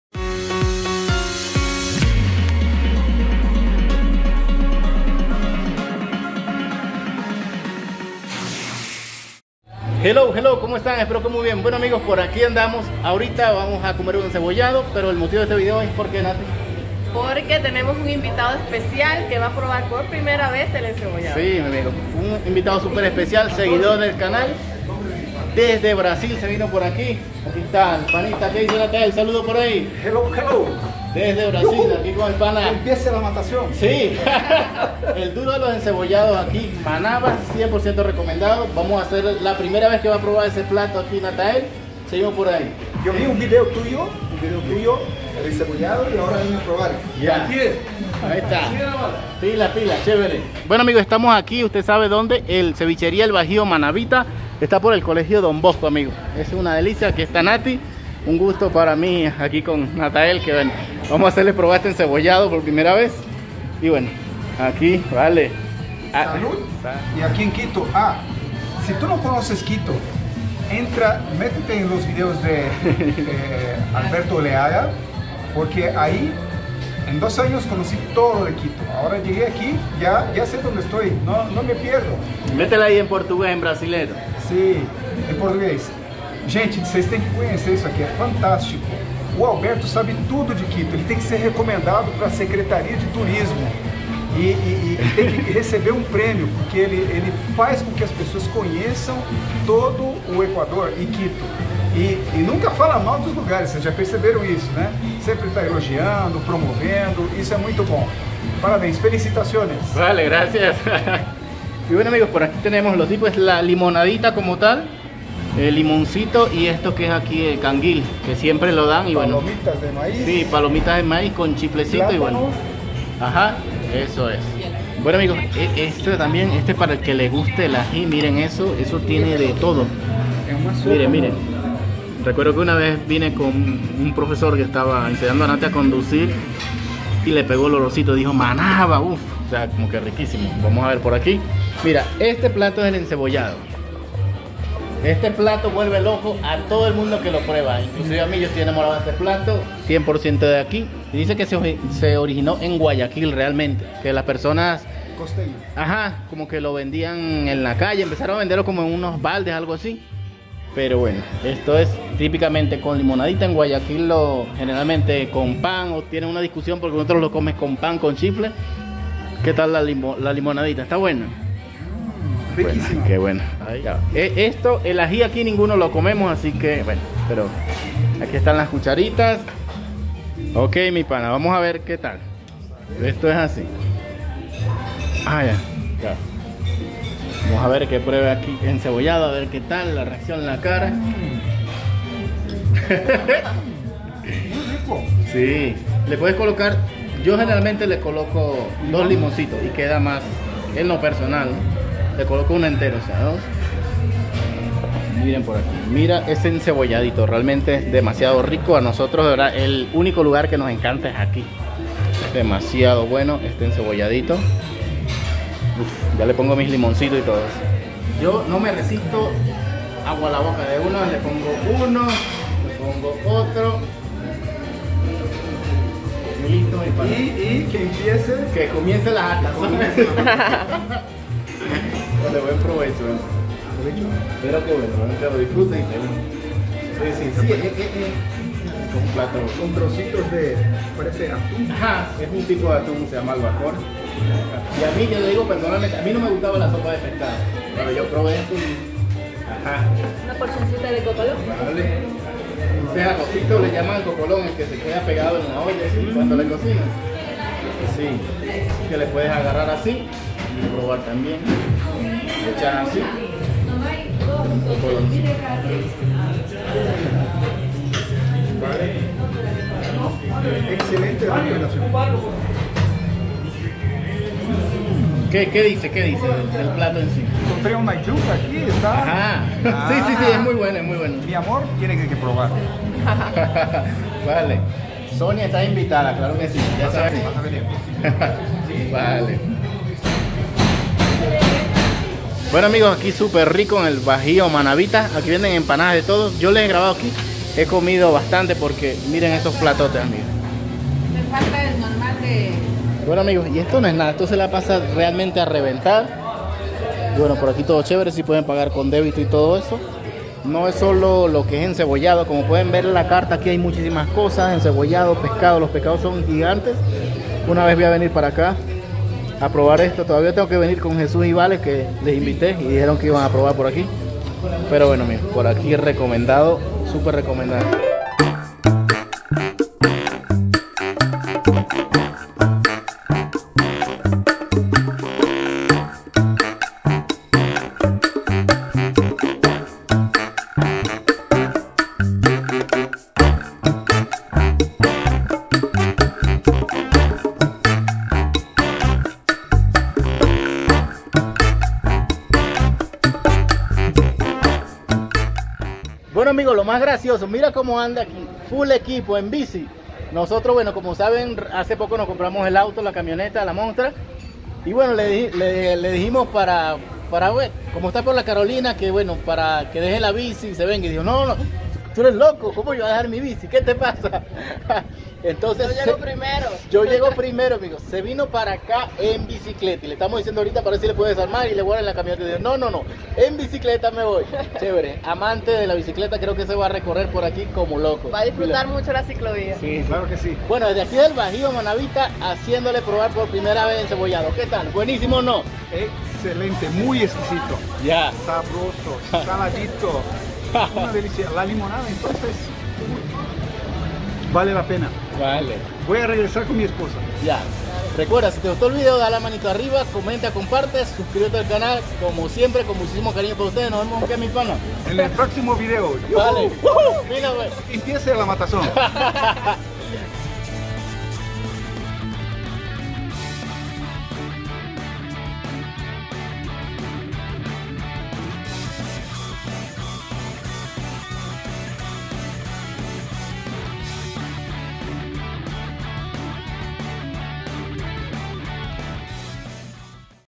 Brasileño prueba Encebollado - Comida Ecuatoriana ¿Le gustó? . Albert Oleaga. Ecuador
Le hago probar la delicia del Encebollado Ecuatoriano a mi amigo  Brasileño , le ha encantado.

Mi nombre es Albert.

Muchas gracias